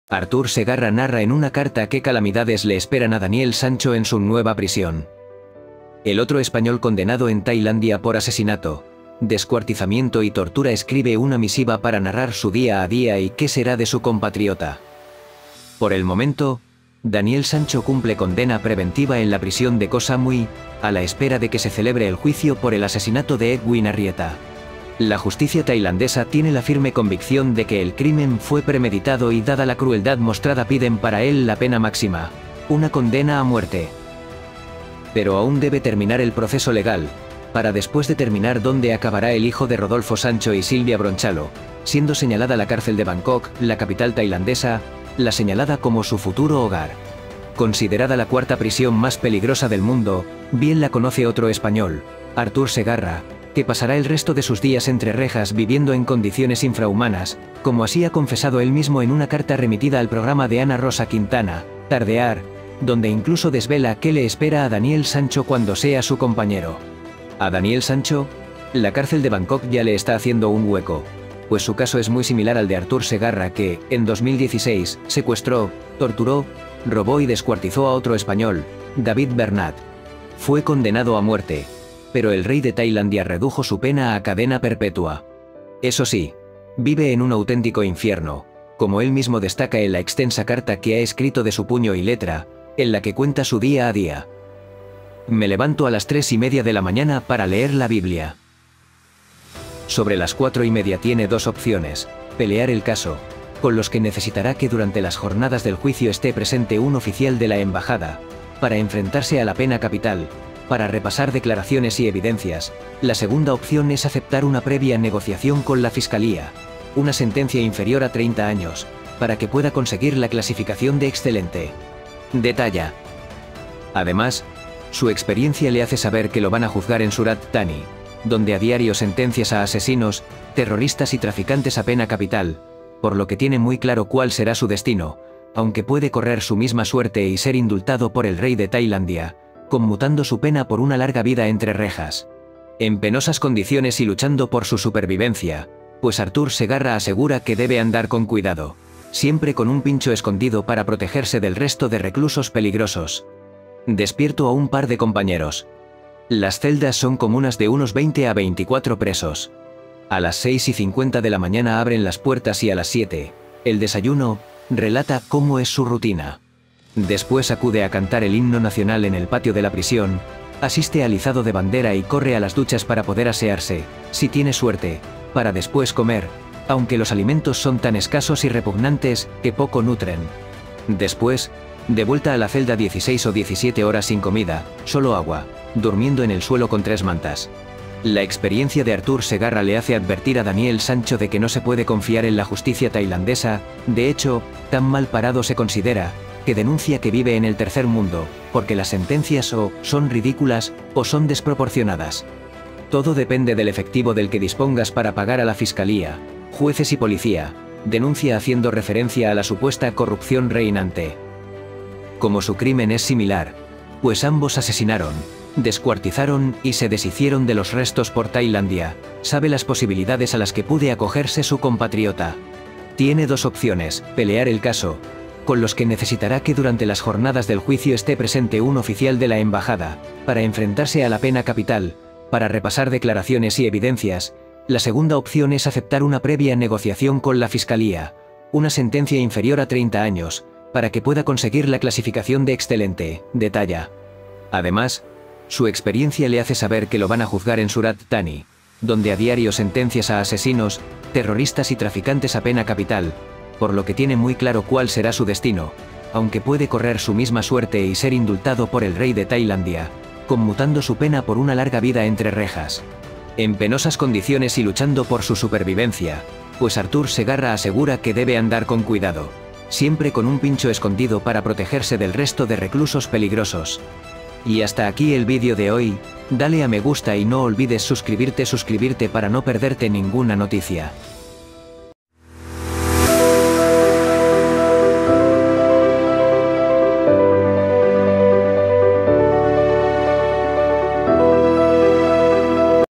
¡ARTHUR SEGARRA CARTA A DANIEL SANCHO! | ÚLTIMA HORA | NEWS
Artur Segarra narra en una carta qué calamidades le esperan a Daniel Sancho en su nueva prisión.
El otro español condenado en Tailandia por asesinato, descuartizamiento y tortura escribe una misiva para narrar su día a día y qué será de su compatriota.

Por el momento, Daniel Sancho cumple condena preventiva en la prisión de Koh Samui, a la espera de que se celebre el juicio por el asesinato de Edwin Arrieta. La justicia tailandesa tiene la firme convicción de que el crimen fue premeditado y dada la crueldad mostrada piden para él la pena máxima, una condena a muerte. Pero aún debe terminar el proceso legal, para después determinar dónde acabará el hijo de Rodolfo Sancho y Silvia Bronchalo, siendo señalada la cárcel de Bangkok, la capital tailandesa, la señalada como su futuro hogar.
Considerada la cuarta prisión más peligrosa del mundo, bien la conoce otro español, Artur Segarra, que pasará el resto de sus días entre rejas viviendo en condiciones infrahumanas, como así ha confesado él mismo en una carta remitida al programa de Ana Rosa Quintana, 'Tardear', donde incluso desvela qué le espera a Daniel Sancho cuando sea.
A Daniel Sancho,  La cárcel de Bangkok ya le está haciendo un hueco, pues su caso es muy similar al de Artur Segarra que, en 2016, secuestró, torturó, robó y descuartizó a otro español, David Bernat. Fue condenado a muerte, pero el rey de Tailandia redujo su pena a cadena perpetua. Eso sí, vive en un auténtico infierno, como él mismo destaca en la extensa carta que ha escrito de su puño y letra, en la que cuenta su día a día.
"Me levanto a las tres y media de la mañana para leer la Biblia (…) sobre las cuatro y mediaTiene dos opciones: pelear el caso, con los que necesitará que durante las jornadas del juicio esté presente un oficial de la embajada, para enfrentarse a la pena capital, para repasar declaraciones y evidencias; la segunda opción es aceptar una previa negociación con la fiscalía, una sentencia inferior a 30 años, para que pueda conseguir la clasificación de excelente", detalla.
Además, su experiencia le hace saber que "lo van a juzgar en Surat Thani, donde a diario sentencias a asesinos, terroristas y traficantes a pena capital", por lo que tiene muy claro cuál será su destino, aunque puede correr su misma 'suerte' y ser indultado por el rey de Tailandia, conmutando su pena por una larga vida entre rejas, en penosas condiciones y luchando por su supervivencia, pues Artur Segarra asegura que debe andar con cuidado, siempre con "un pincho" escondido para protegerse del resto de reclusos peligrosos. despierto a un par de compañeros. Las celdas son comunas de unos 20 a 24 presos. A las seis y cincuenta de la mañana abren las puertas y a las siete, el desayuno", relata cómo es su rutina.
 Después acude a cantar el himno nacional en el patio de la prisión, asiste al izado de bandera y corre a las duchas para poder asearse, si tiene suerte, para después comer, aunque los alimentos son tan escasos y repugnantes, que poco nutren. Después, de vuelta a la celda "16 o 17 horas sin comida, solo agua, durmiendo en el suelo con tres mantas".

La experiencia de Artur Segarra le hace advertir a Daniel Sancho de que "no se puede confiar en la justicia tailandesa". De hecho, tan mal parado se considera, que denuncia que vive en "el tercer mundo, porque las sentencias o, son ridículas, o son desproporcionadas. Todo depende del efectivo del que dispongas para pagar a la fiscalía, jueces y policía", denuncia haciendo referencia a la supuesta corrupción reinante. Como su crimen es similar, pues ambos asesinaron, descuartizaron y se deshicieron de los restos por Tailandia, sabe las posibilidades a las que pude acogerse su compatriota:

"Tiene dos opciones: pelear el caso, con los que necesitará que durante las jornadas del juicio esté presente un oficial de la embajada, para enfrentarse a la pena capital, para repasar declaraciones y evidencias; la segunda opción es aceptar una previa negociación con la fiscalía, una sentencia inferior a 30 años, para que pueda conseguir la clasificación de excelente", detalla. Además, su experiencia le hace saber que "lo van a juzgar en Surat Thani, donde a diario sentencias a asesinos, terroristas y traficantes a pena capital", por lo que tiene muy claro cuál será su destino, aunque puede correr su misma 'suerte' y ser indultado por el rey de Tailandia, conmutando su pena por una larga vida entre rejas, en penosas condiciones y luchando por su supervivencia, pues Artur Segarra asegura que debe andar con cuidado, siempre con "un pincho" escondido para protegerse del resto de reclusos peligrosos.